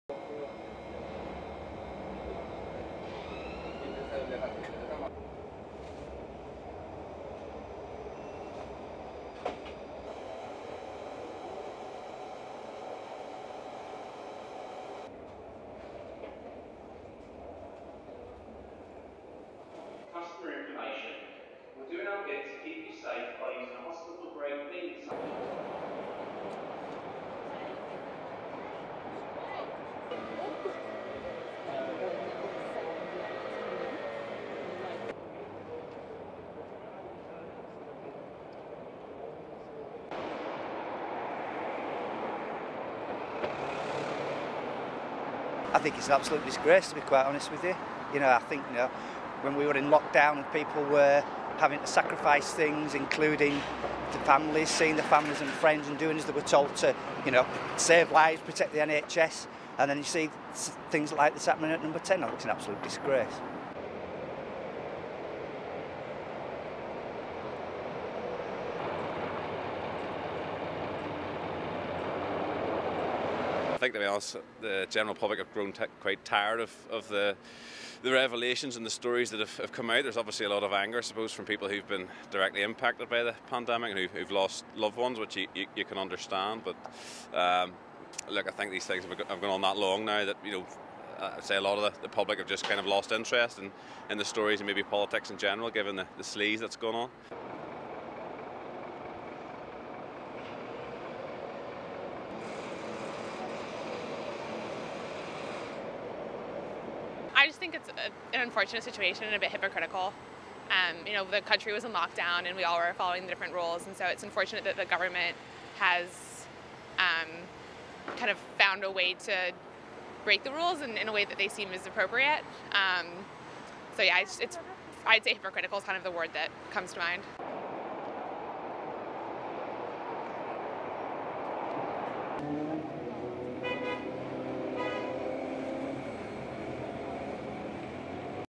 Reax as details of more Downing St events emerge
(14 Jan 2022) Britons on Friday described revelations of further parties taking place at Number 10 Downing Street during lockdown periods as an "absolute disgrace" and "hypocritical".
Pressure on Boris Johnson grew Friday after a newspaper published allegations of two more parties by staff in the prime minister's office, complete with drinking, music and dancing.
The Daily Telegraph said the leaving parties for two staffers took place in April 2021, the night before the funeral of 99-year-old Prince Philip, the husband of Queen Elizabeth II.
The queen sat alone in the church during the service in order to adhere to social distancing rules that barred indoor mixing.
In Westminster members of the public expressed their disappointment over the actions of top civil servants.
"When we were in lockdown and people were having to sacrifice things, including seeing their families and friends and doing as they were told to, you know, save lives, protect the NHS, and then you see things like this happening at Number 10, I think it's an absolute disgrace," said Stuart Walton, a civil servant from Leeds.
Bank worker Jamie Salzman labelled the events "an unfortunate situation and a bit hypocritical".
But 29-year-old banker Adam Simpson from Carrickfergus in Northern Ireland feared the public had grown quite tired of the revelations and had lost interest in politics more generally.
The new revelation came after Johnson apologised in the House of Commons on Wednesday for attending a “bring your own booze” party in the garden of the prime minister’s Downing Street office and residence in May 2020.
About 100 staff were invited by a senior prime ministerial aide to what was billed as a “socially distanced drinks” event.